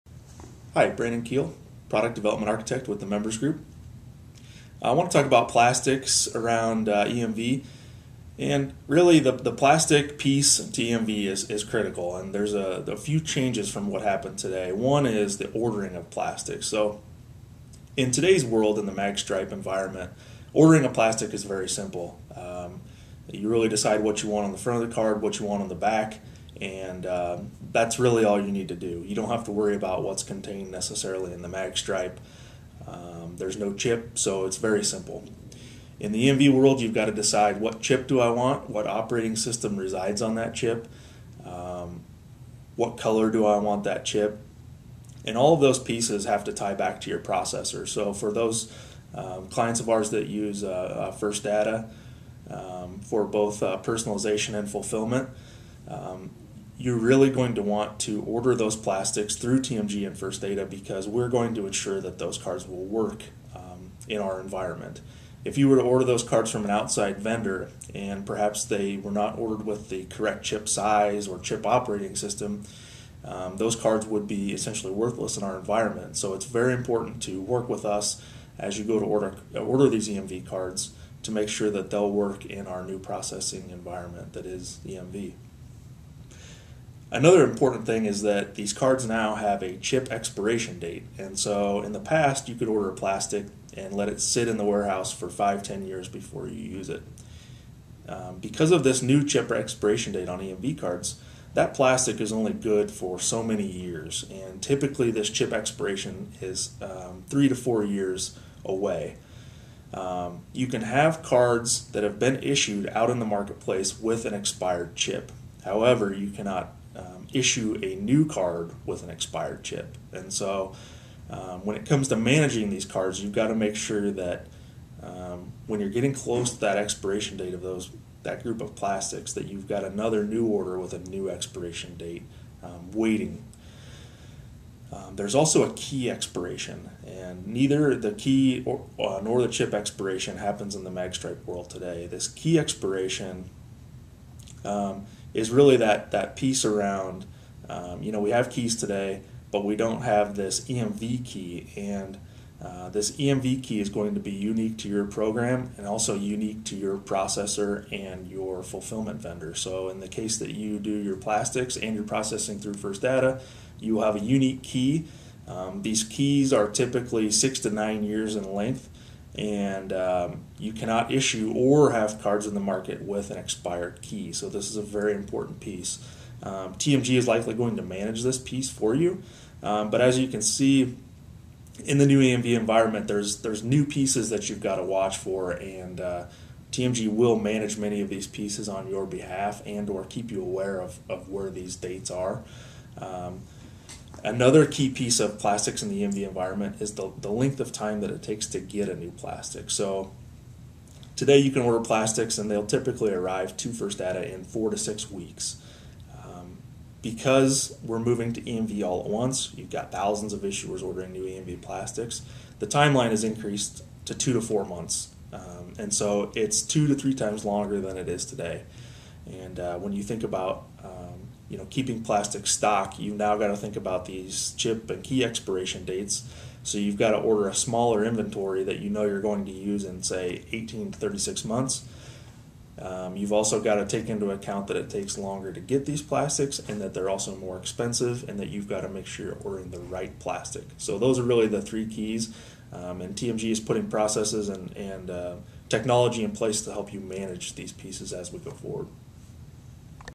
EMV Video Update - Ordering & Managing EMV Plastics
In this brief video, TMG Product Development Architect Brandon Kuehl provides information on ordering and managing EMV plastics. In today's world, ordering new plastics is simple. With EMV, there are several items to be mindful of with chip technology, maintenance and expiration dates. Brandon discusses the pieces for ordering EMV cards and how TMG will help your financial institution manage the process.